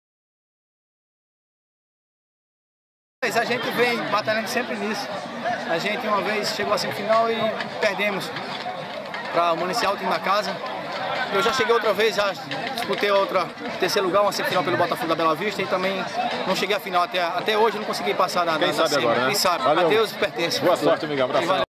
Goleiro Dassayev sonhando em chegar na grande final da Copa Primo Fernandes
Uma lenda viva do futebol amador, sempre levado a melhor nas penalidades o goleiro Dassayev que já disputou duas semifinais sonha em chegar pela primeira vez a grande final